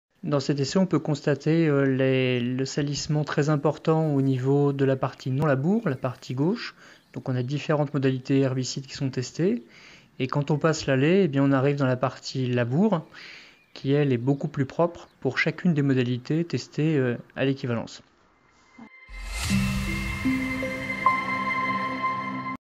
Comparaison labour et non labour : essai VALFRANCE agronomie et désherbage chimique sur ray grass
Dans cet essai sur blé, on peut constater le salissement en ray grass de la partie en non labour (partie gauche), sur différentes modalités herbicides testées. En passant l'allée, nous arrivons dans la partie labour (semée le même jour) qui, elle, est beaucoup plus propre pour les mêmes modalités herbicides appliquées. Commentaires de la vidéo par Alain LALOI, responsable technique Val'Epi et expert du Pôle Agronomie EASI'NOV.
Essai VALFRANCE agronomie et désherbage chimique sur ray grass dans du blé - plateforme de Moisenay (77) - juin 2017.